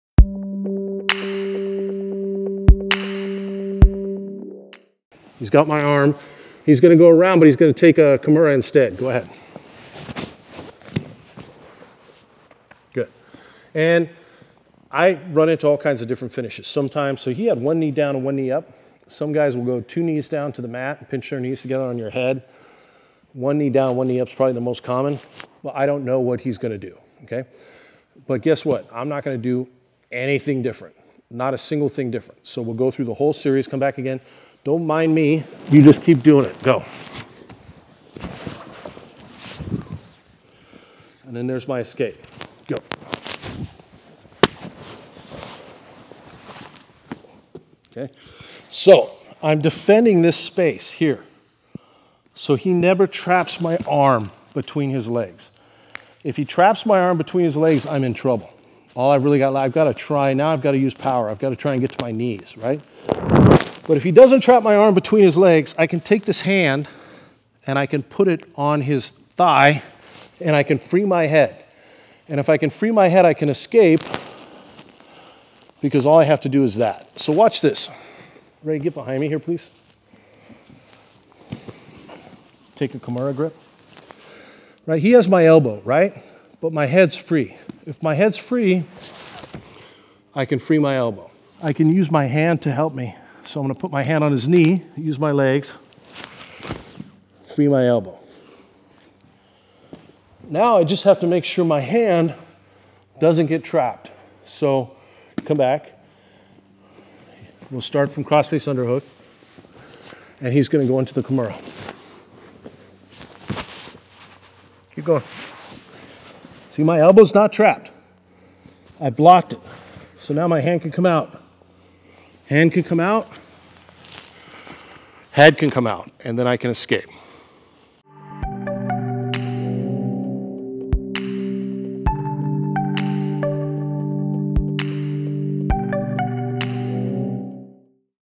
One Way to Survive Multiple Submissions in BJJ • Survive and Escape
In this video coach Matt Thornton shows how a single escape can help you survive multiple submissions in BJJ.